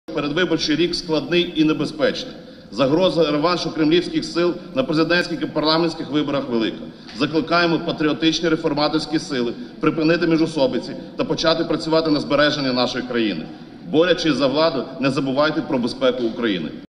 "Народний фронт" закликає демократичні сили припинити міжусобиці
Керівник фракції "Народний фронт" Максим Бурбак закликав парламентські демократичні сили припинити міжусобиці та почати працювати для збереження країни.

Про це він сказав під час виступу на Погоджувальній раді.

"Передвиборчий рік складний і небезпечний. Загроза реваншу кремлівських сил на президентських і парламентських виборах велика. Закликаємо патріотичні реформаторські сили припинити міжусобиці та почати працювати на збереження нашої країни, - підкреслив Максим Бурбак.

"Борячись за владу – не забувайте про безпеку України!", - наголосив керівник фракції "Народний фронт".

Приєднуйся!